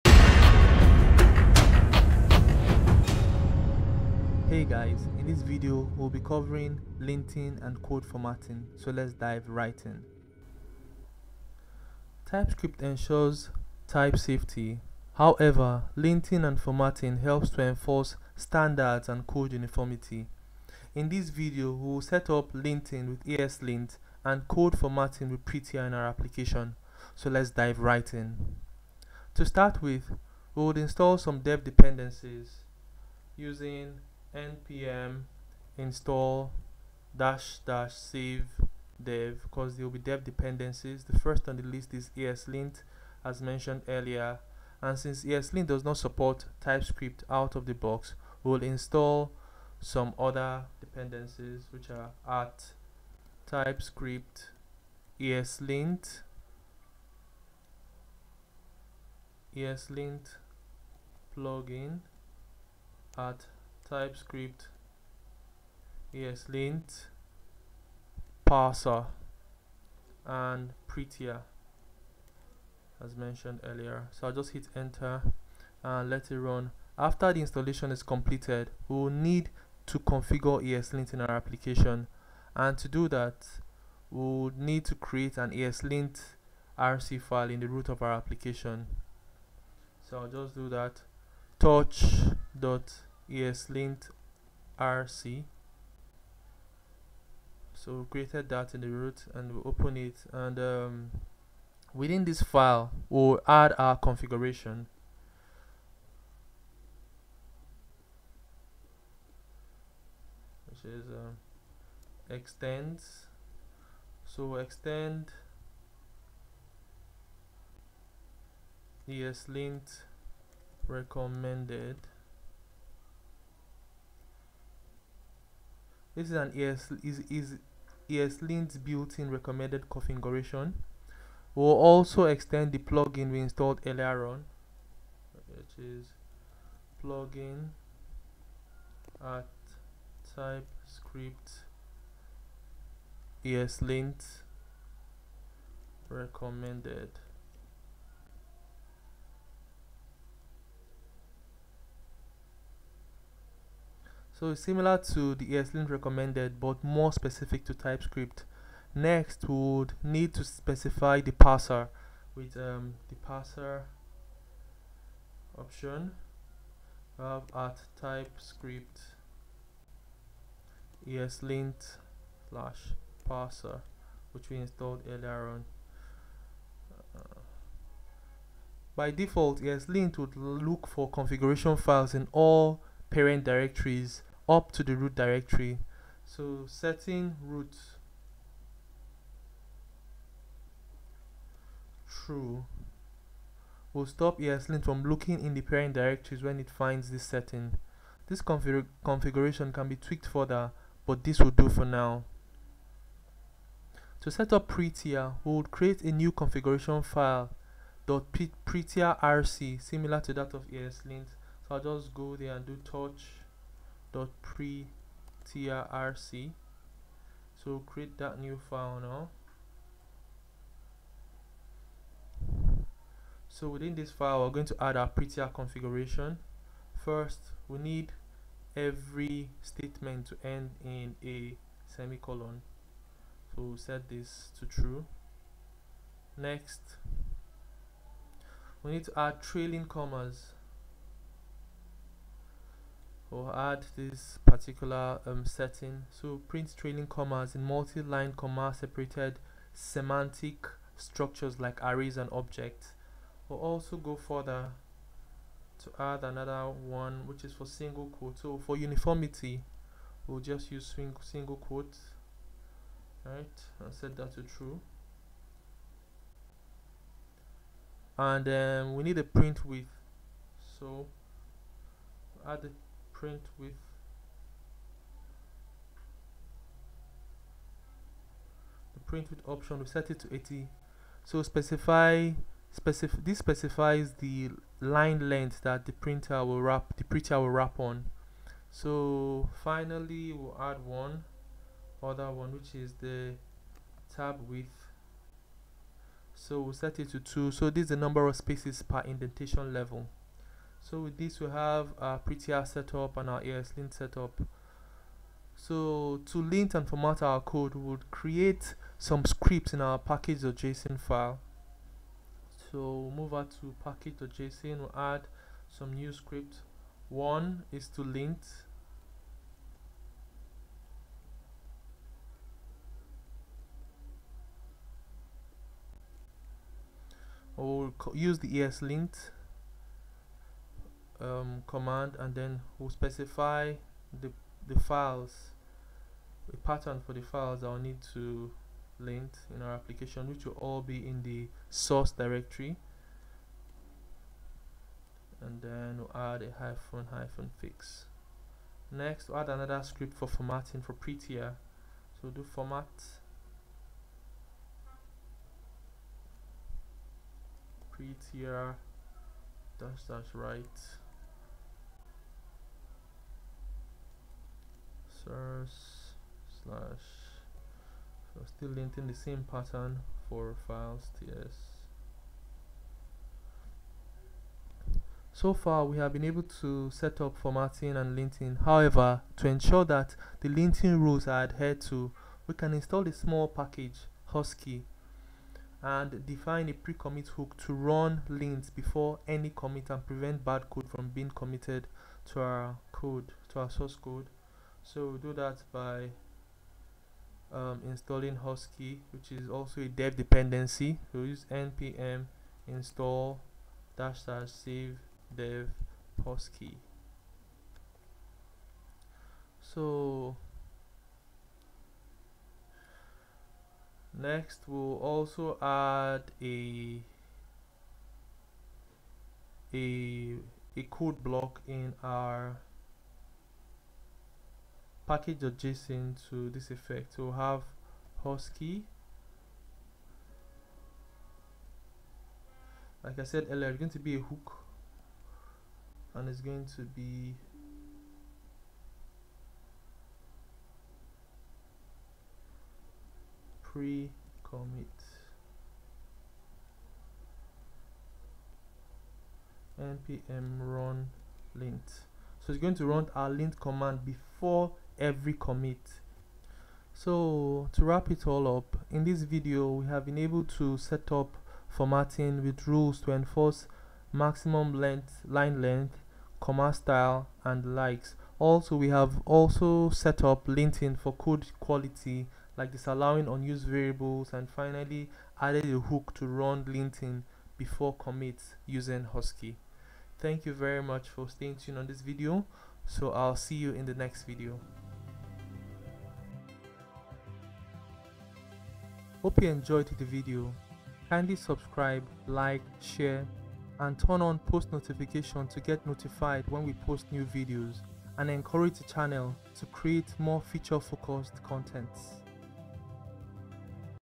#2 Setup linting and code formatting
Payment systems are built pretty much alike with almost the same flow.

- Generate a secure payment authorization link.
- Direct users to the generated link to authorize payment through card or bank payment authorization details.
- Verify payment using the payment reference from the previous step.
- Provide value for verified payment.

This video covers linting and formatting using eslint, prettier and husky for the API payment integration app.